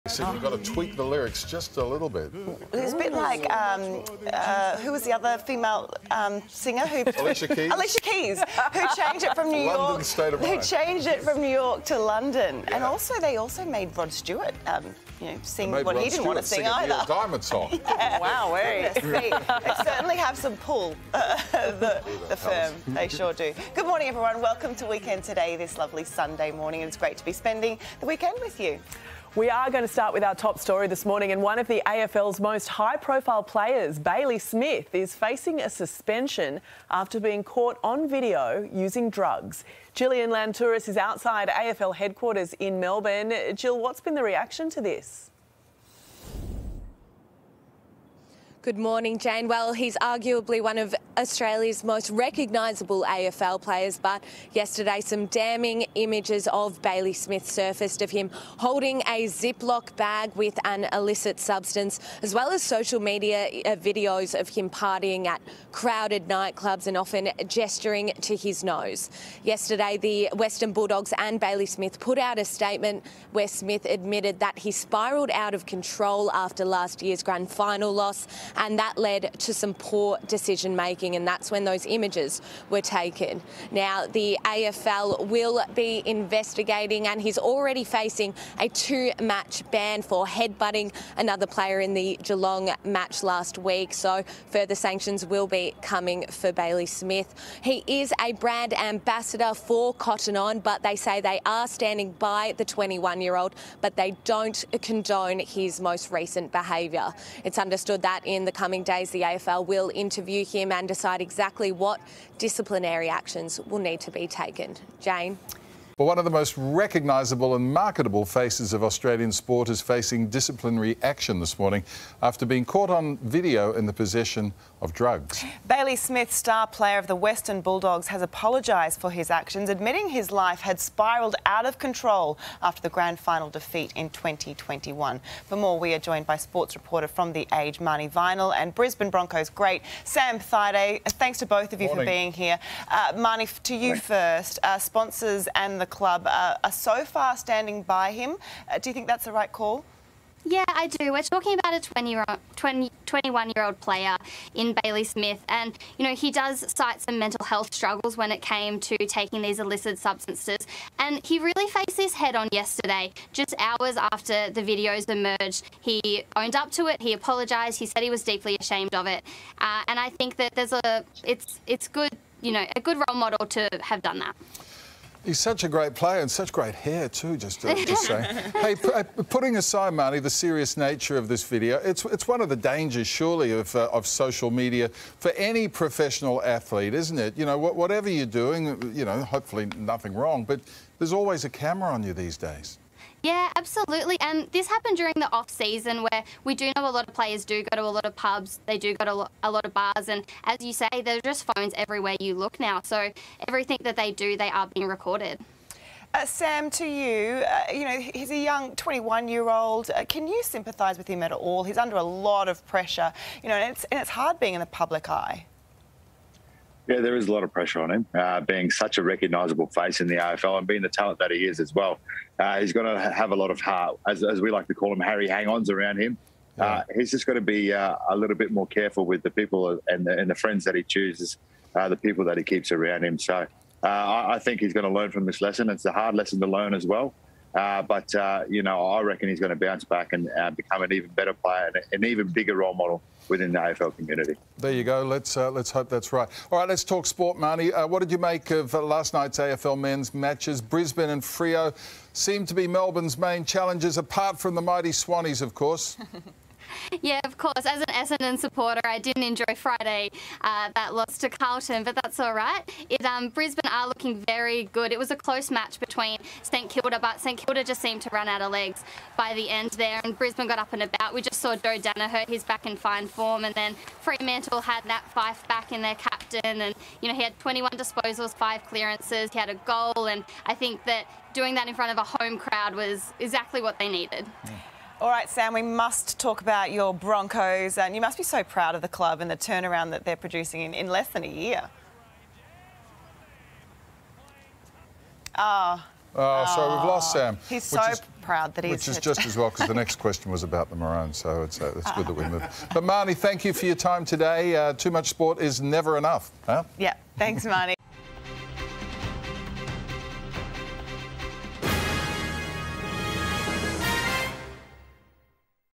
AFL star Bailey Smith's confession over illicit drug scandal | 9 News Australia
One of AFL’s most high profile players Bailey Smith, 21, from the Western Bulldogs is facing a suspension after admitting he used illicit drugs while caught on video.